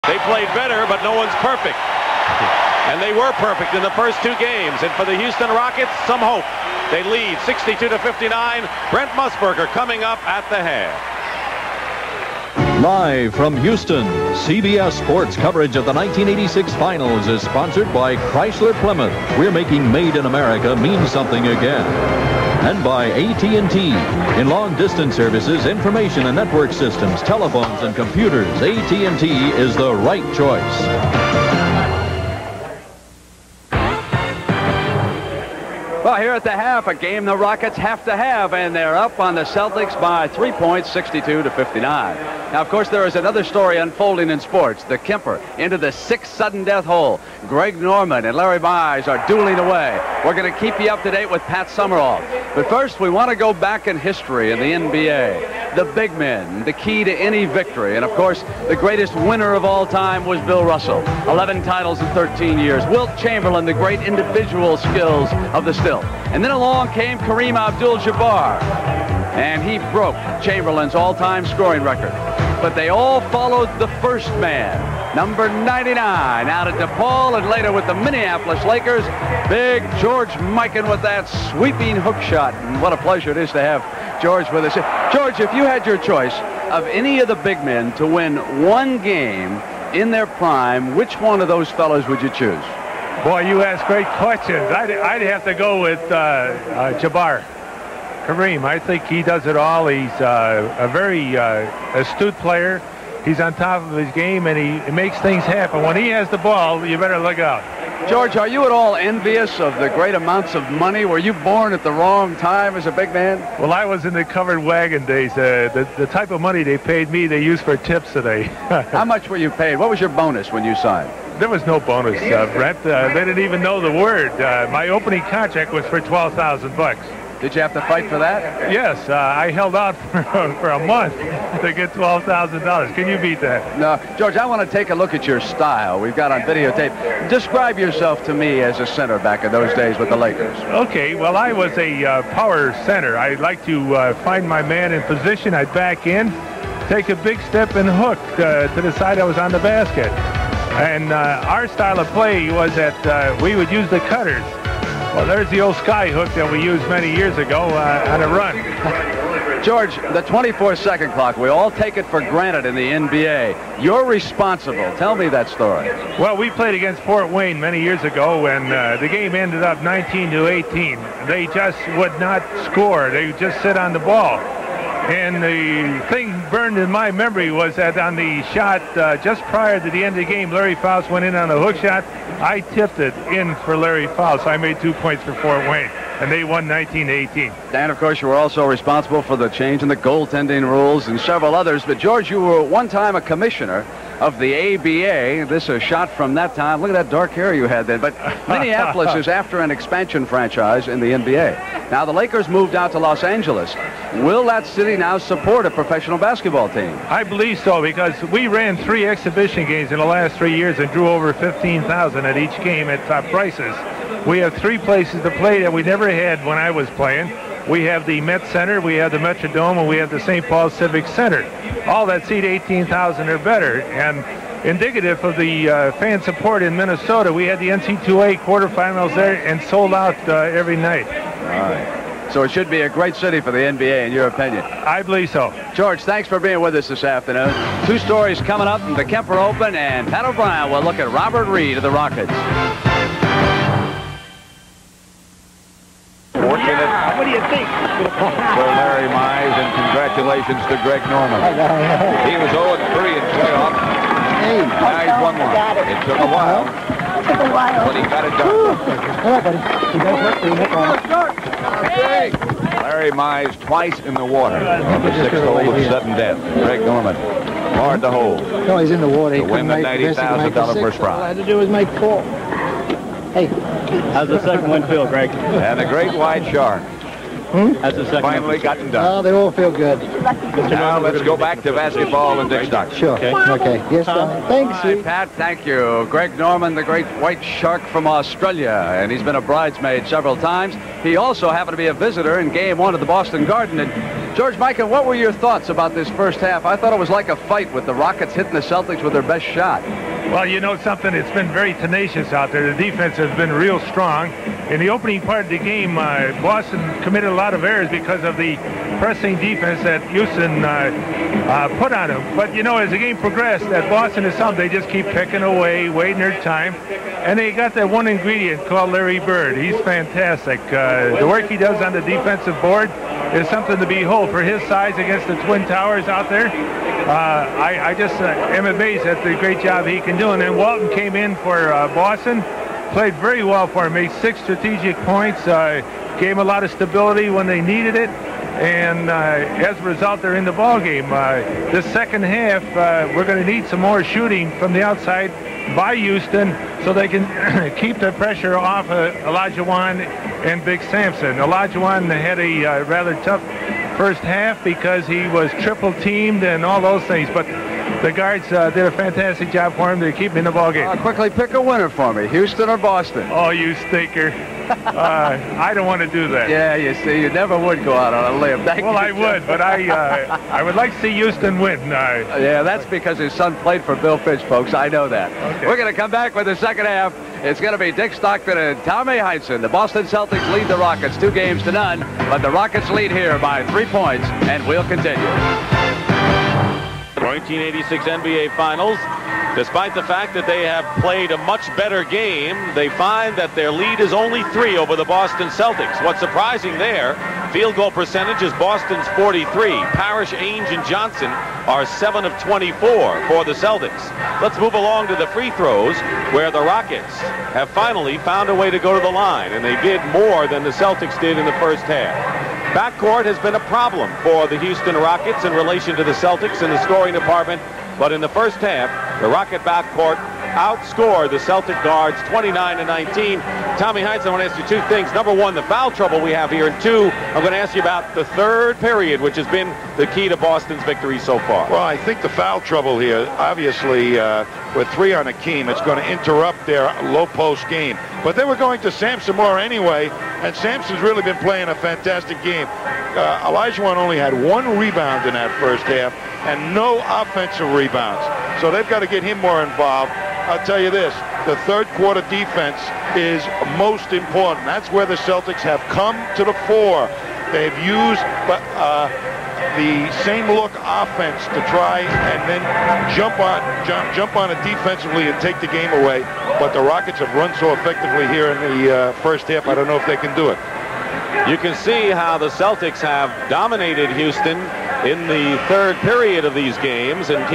NBA - 1986 - NBA Finals Game 3 Celtics Vs Rockets Halftime Show With Brent Musburger + George Mikan
has the largest vintage basketball video library online